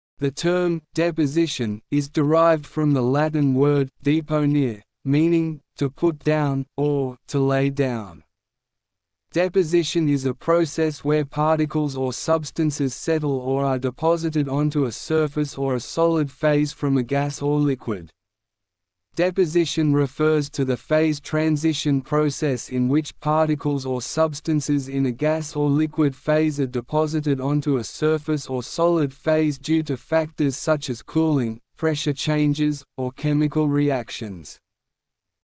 Deposition Meaning | Definition Deposition and its etymology
Decoding Deposition: Meaning, Etymology, and Definition"
 Join us on an enlightening journey as we decode the meaning, etymology, and definition of deposition. Explore the intriguing process where particles or substances settle onto surfaces or solid phases from gases or liquids. Uncover the historical origins and scientific principles behind deposition, and discover its significance in geology, material science, and atmospheric phenomena.

 deposition, meaning of deposition, etymology, definition, process, particles, substances, surfaces, solid phases, geology, material science, atmospheric phenomena.